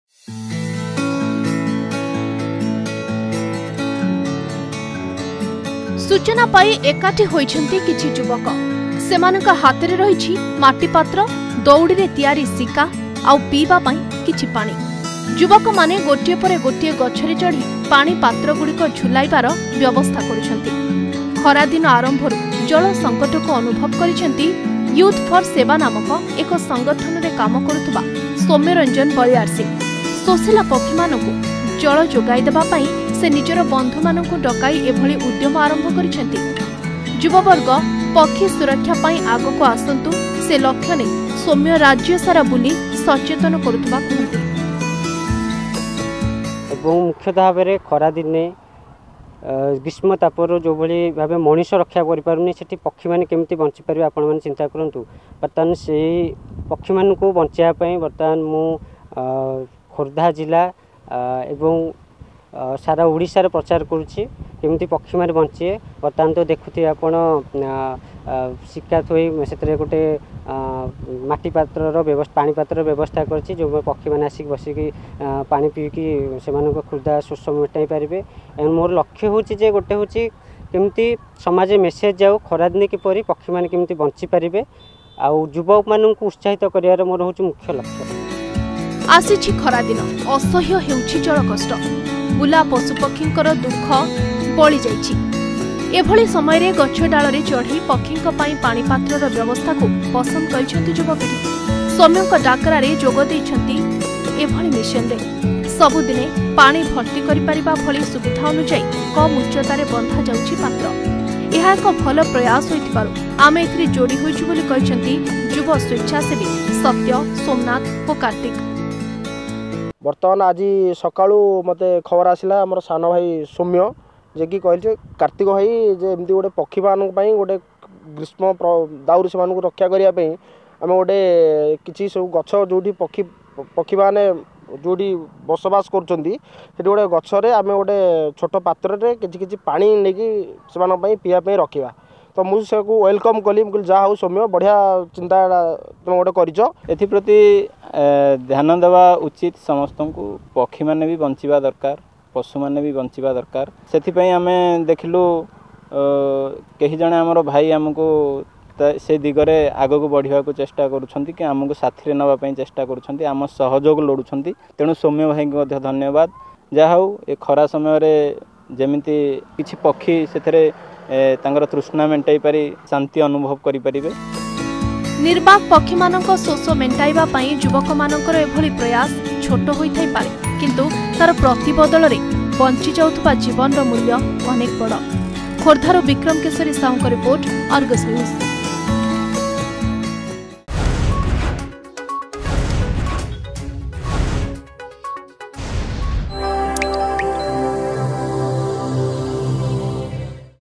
ଗିନାଏ ପାଣି ବଞ୍ଚାଇ ଦେବ ଜୀବନ
ଖରାଦିନ ଆରମ୍ଭରୁ ଜଳ ସଙ୍କଟକୁ ଅନୁଭବ କରିଛନ୍ତି ୟୁଥଫରସେବା ନାମକ ଏକ ସଂଗଠନରେ କାମ କରୁଥିବା ସୌମ୍ୟ ରଞ୍ଜନ ବଳିୟାରସିଂ । ଶୋଷିଲା ପକ୍ଷୀ ମାନଙ୍କୁ ଜଳ ଯୋଗାଇଦେବା ପାଇଁ ସେ ନିଜର ବନ୍ଧୁ ମାନଙ୍କୁ ଡ଼କାଇ ଏଭଳି ଉଦ୍ୟମ ଆରମ୍ଭ କରିଛନ୍ତି...